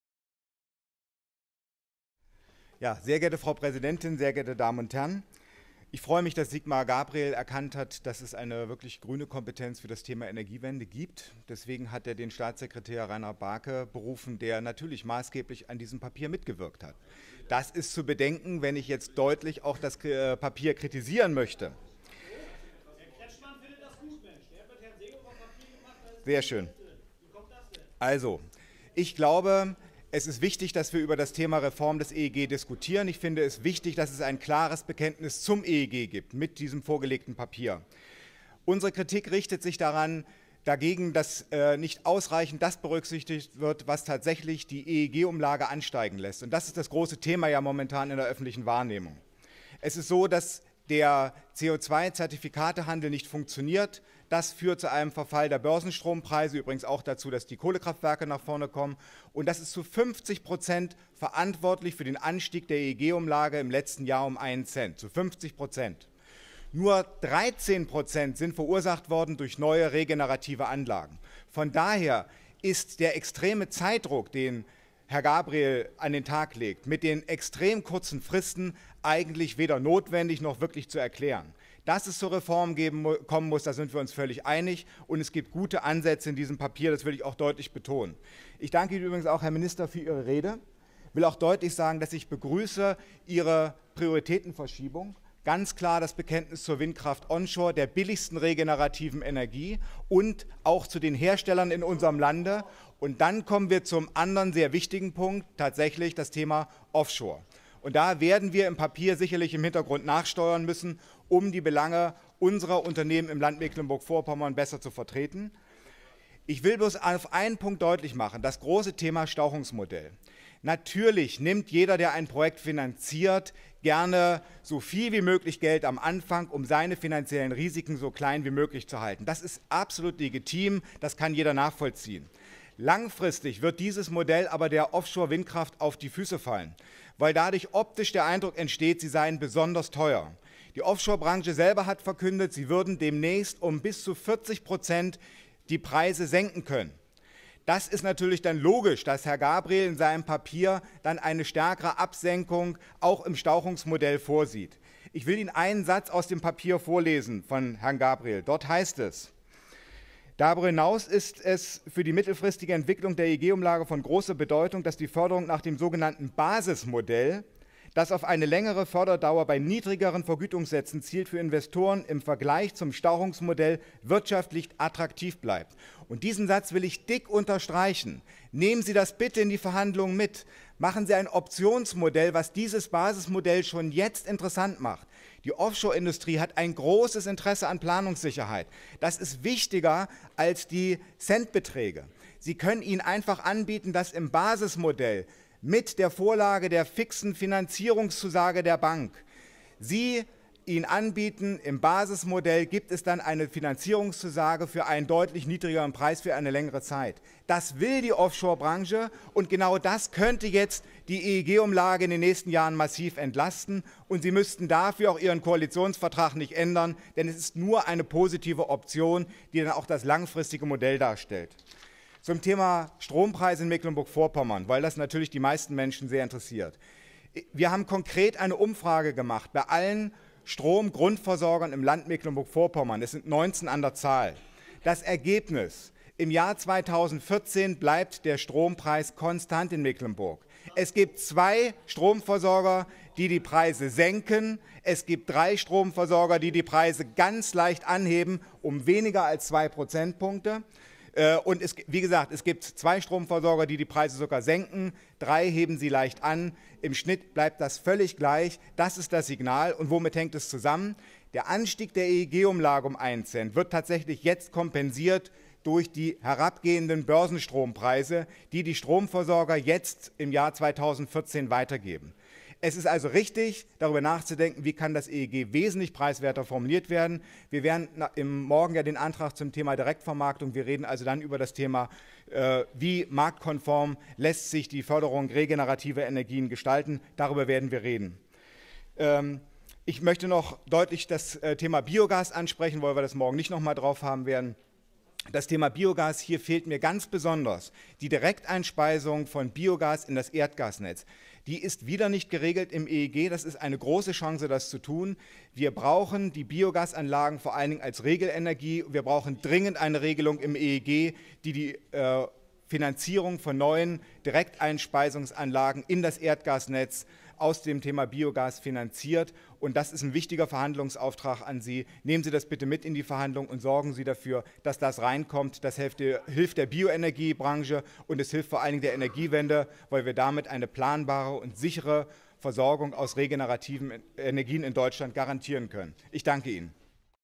Rede von Johann-Georg Jaeger zur Aktuellen Stunde
Dies ist die Rede von Johann-Georg Jaeger zur Aktuellen Stunde "Energiewende sicher und bezahlbar gestalten".

Quelle: Landtag Mecklenburg-Vorpommern